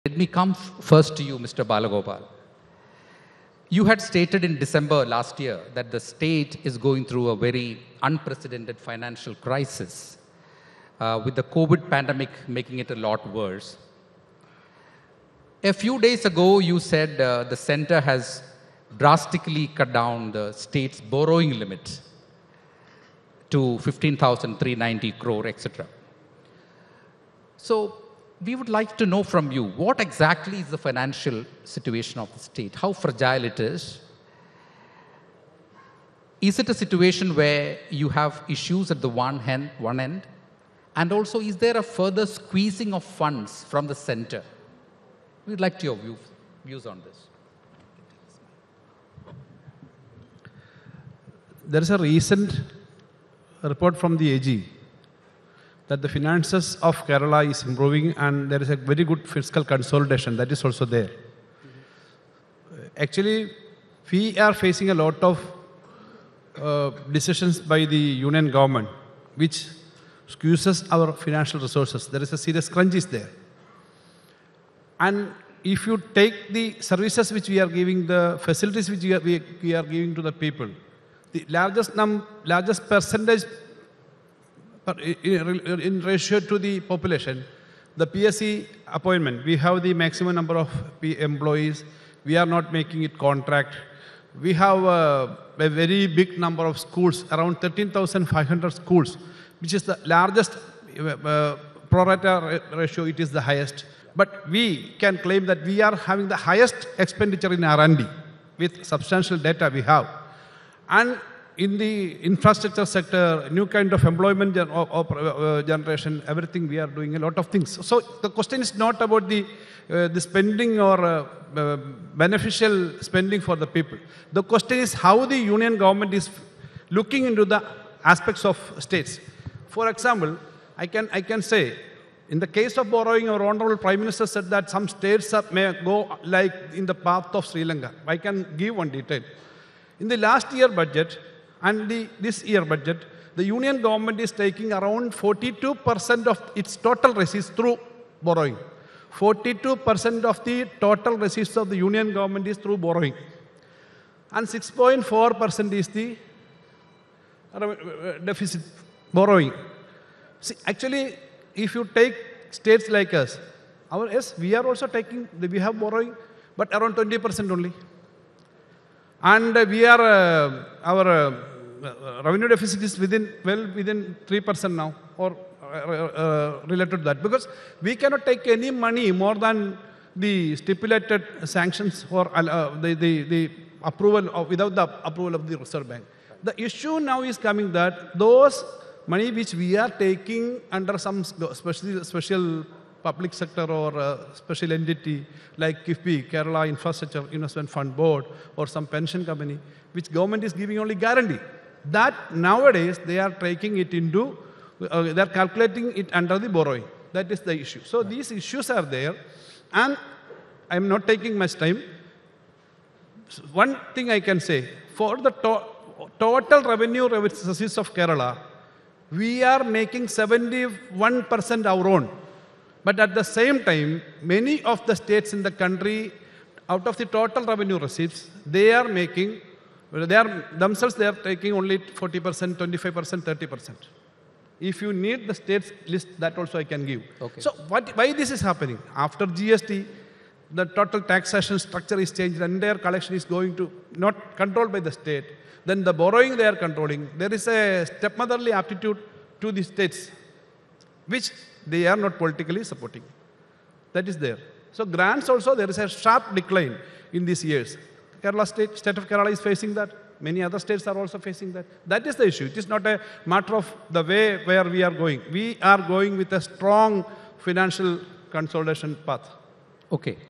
Centre Doesn't Give A Penny It Earns From Cess, Surcharge Collections To States: Kerala FM
Kerala Finance Minister KN Balagopal, while speaking about the Centre's finance policies, said that earlier Union government used to earn 10 per cent from cess and surcharges, but now it's 20 per cent (around Rs 5.5 lakh crores) but they are not giving a single penny to any of the states.